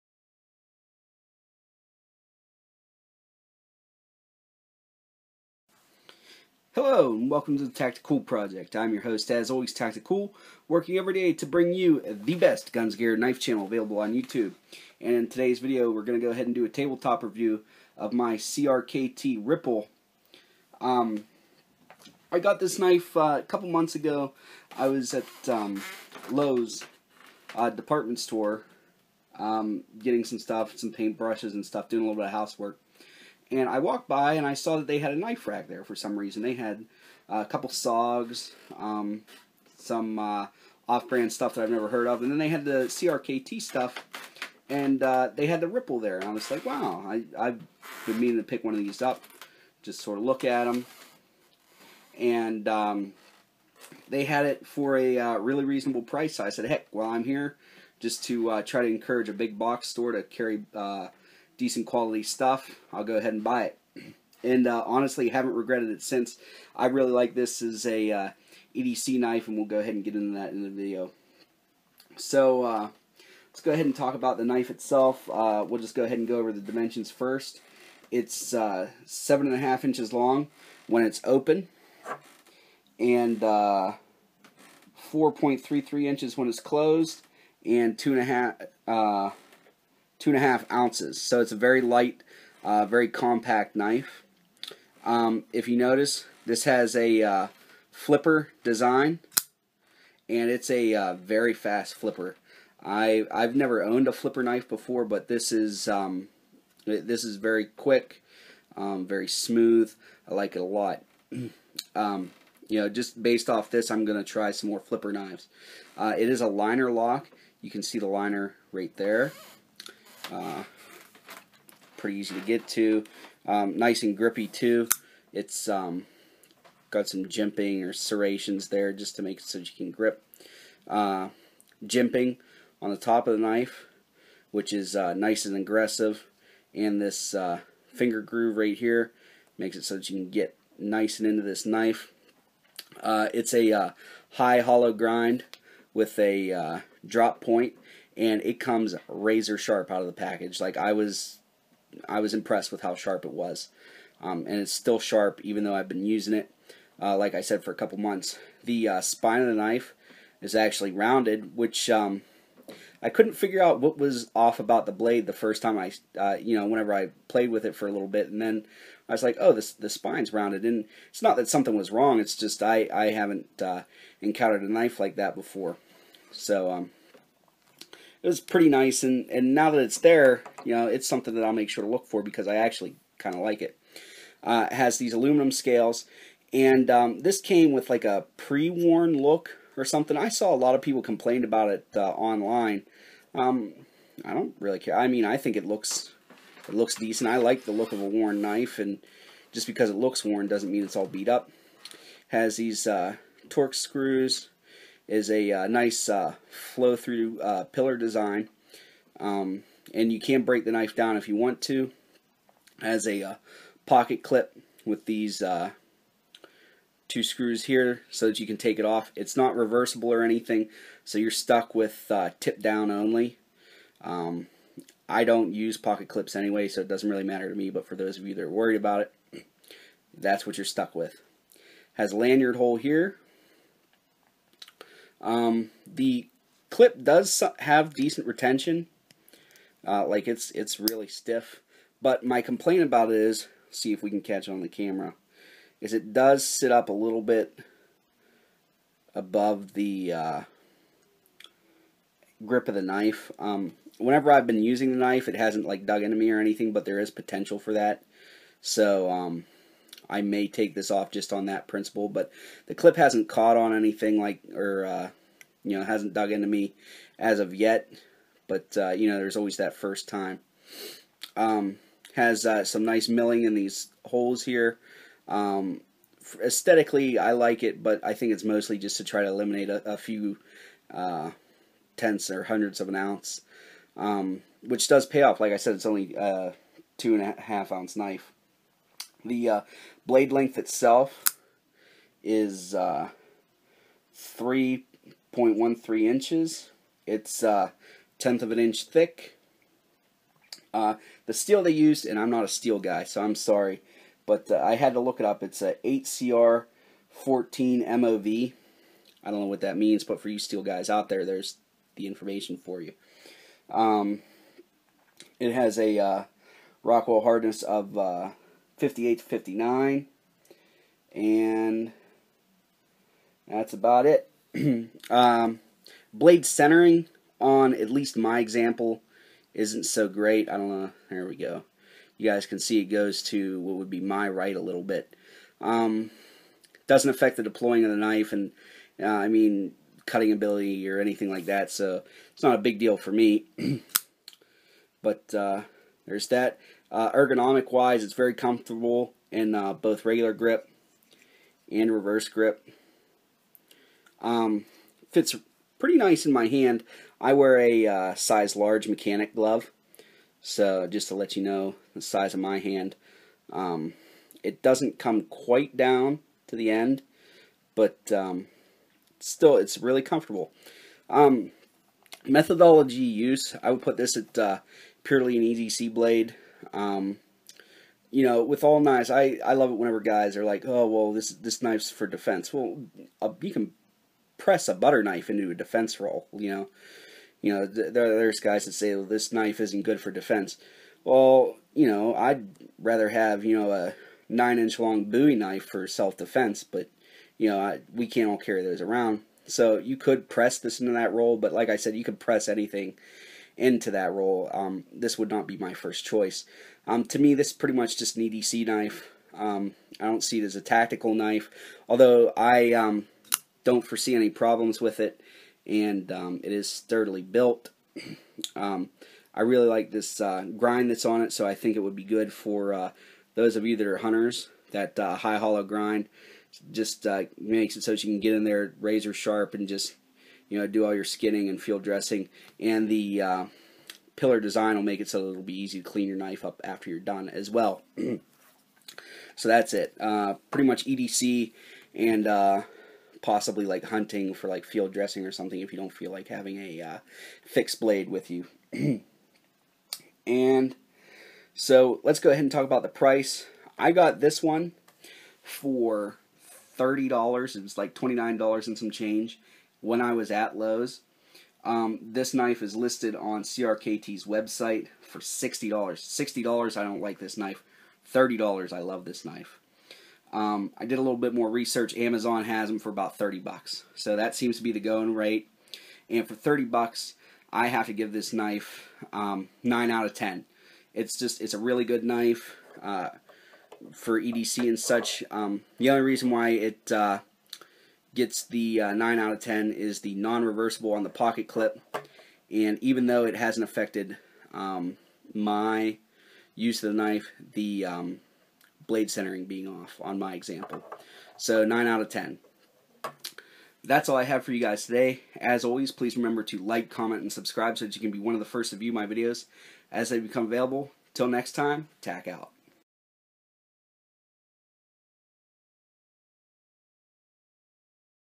CRKT Ripple review | Finding a gem at a big box store.
Hello, and welcome to the Tacticool Project, working everyday to bring you the best guns, gear, and knife channel available on YouTube.  Today's video is a table to review of my CRKT Ripple.  This ended up being a great EDC knife and ranked at a 9/10.  Watch the video and find out how.  Until next time, Tac out!